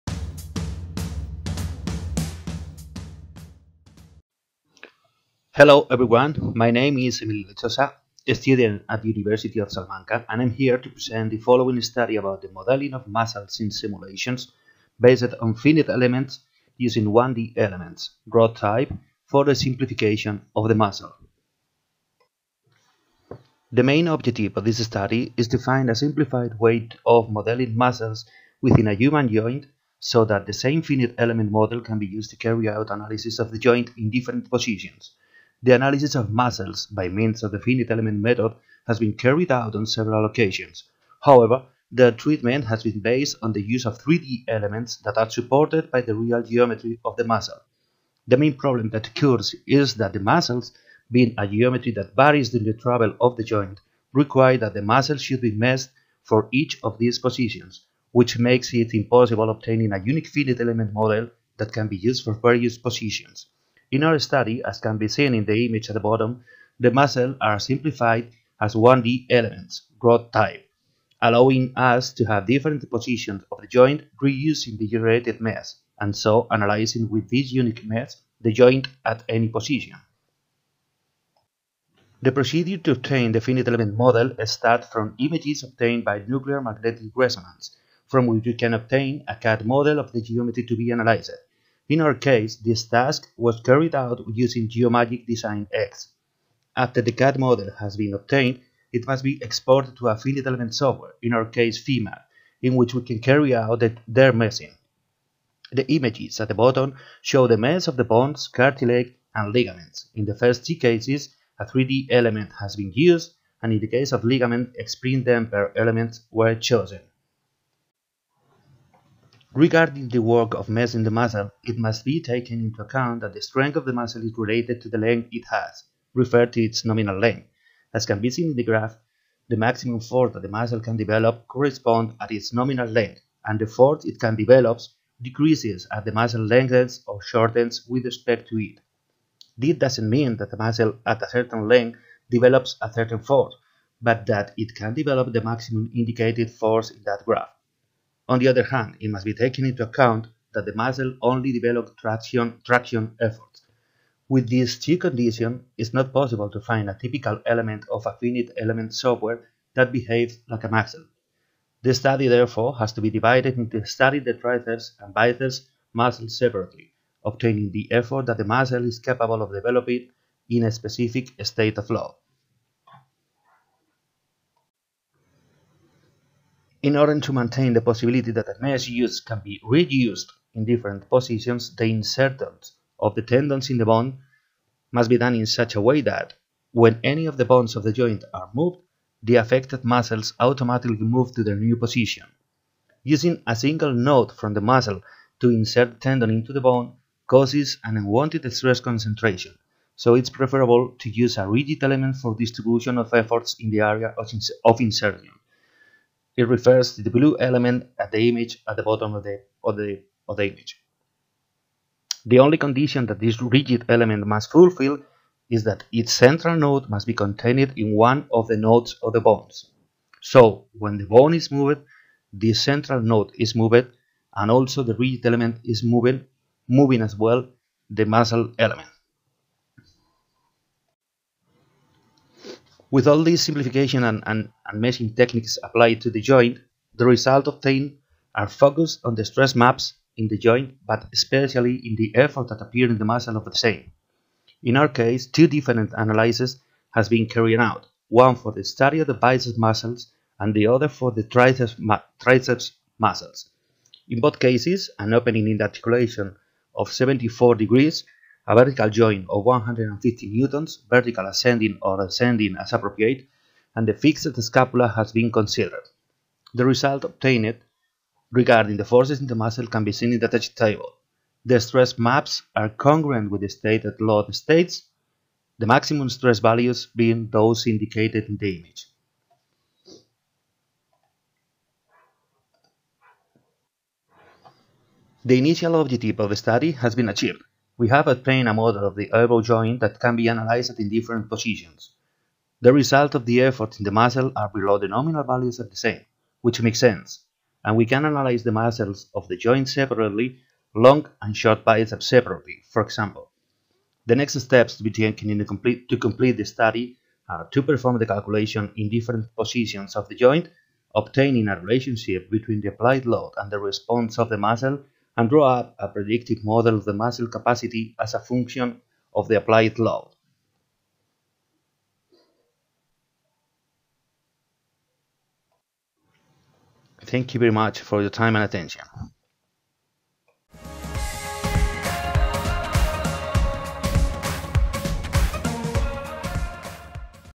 Finite Element model of an elbow under load, muscle effort analysis when modeled using 1D rod eleme
Authors: Emilio Lechosa Urquijo, Fernando Blaya Haro, Roberto D'Amato and Juan Antonio Juanes Méndez

How to cite this work:
Lechosa Urquijo, E., Blaya Haro, F., D'Amato, R. and Juanes Méndez, J. A. (2020). Finite Element model of an elbow under load, muscle effort analysis when modeled using 1D rod eleme. In F. J. García-Peñalvo (Eds.), Proceedings of the 8th International Conference on Technological Ecosystems for Enhancing Multiculturality (TEEM 2020) (Salamanca, Spain, October 21-23, 2020)